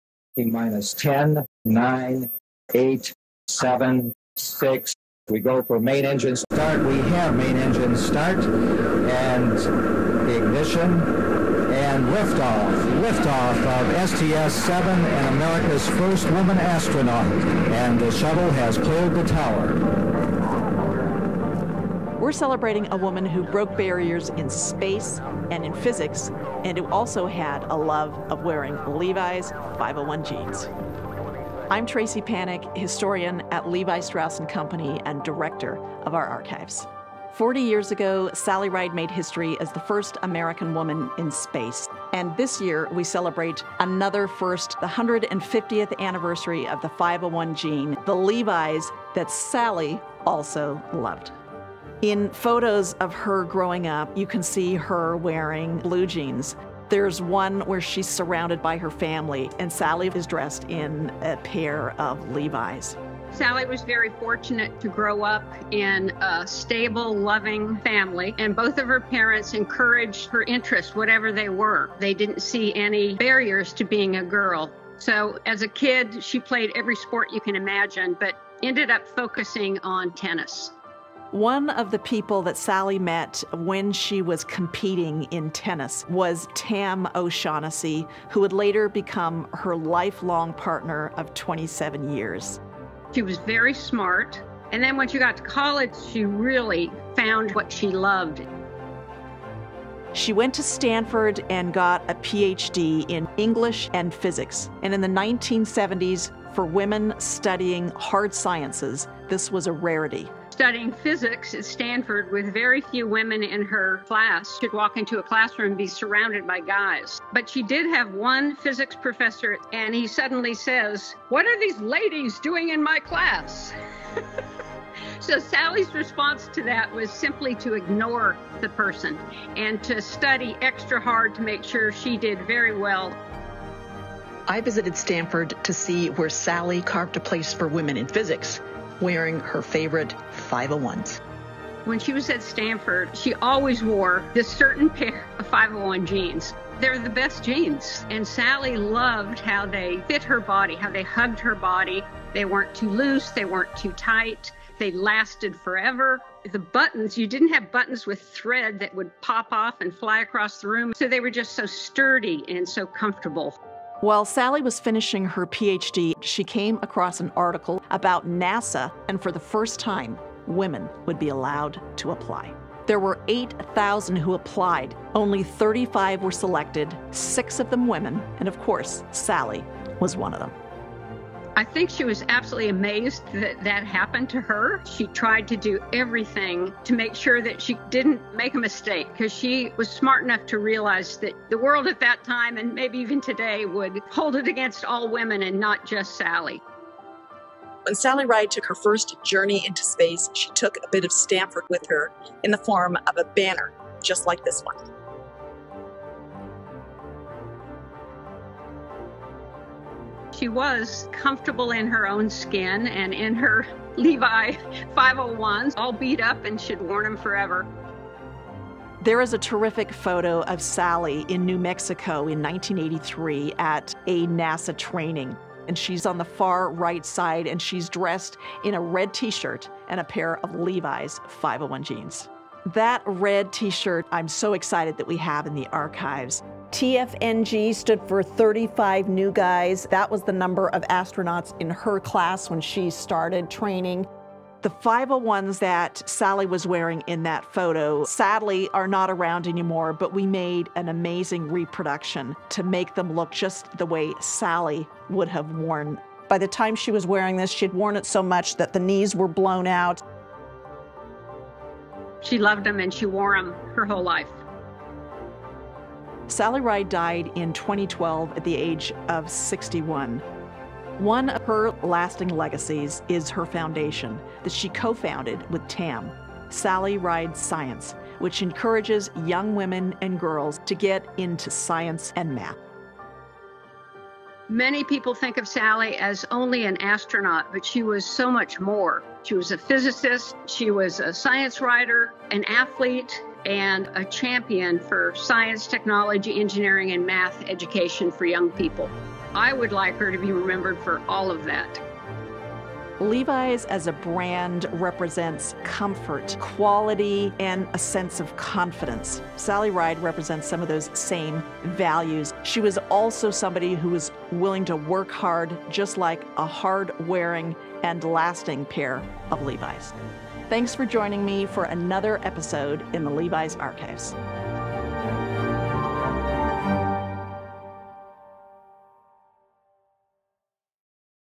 From the Levi’s Archives: Sally Ride - Astronaut, Physicist, and Levi's 501 Fan
Join us in celebrating the incredible journey of Sally Ride, the first American woman in space, as we mark the 40th anniversary of her groundbreaking achievement. Discover the role Levi's® 501® jeans played in Sally's life, representing confidence and freedom as she excelled in physics, tennis, and NASA training.

Sally's cherished Levi's® 501® jeans provided the perfect fit, lasting comfort, and quality she needed. From her early days exploring the night sky to her university years and beyond, these jeans were by her side. When Sally learned about NASA's search for astronauts, she seized the opportunity and became one of the 35 selected out of 8,000 applicants, carrying her trusty 501® jeans to rigorous training in New Mexico.

On June 18th, 1983, Sally made history, shattering barriers and achieving greatness. Although her life was tragically cut short, her legacy lives on through Sally Ride Science, inspiring young women to pursue STEM studies.

Join Levi's® Historian Tracey Panek as we honor Sally Ride's remarkable journey and celebrate the spirit of trailblazing, resilience, and the pursuit of greatness in the Levi’s® Archives.